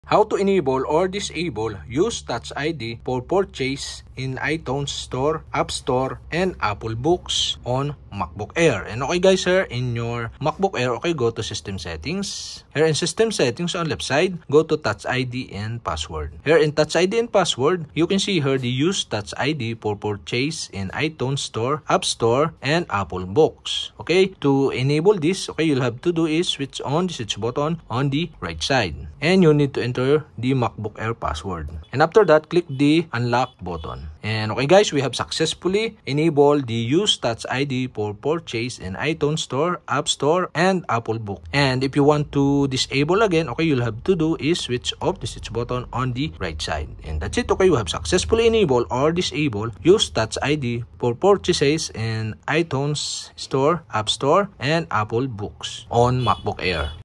How to Enable/Disable use touch id for purchases in itunes store app store and apple books on Mac
In this tutorial i will teach you on how to Enable or Disable use touch id for purchases in itunes store app store and apple books on MACBOOK AIR.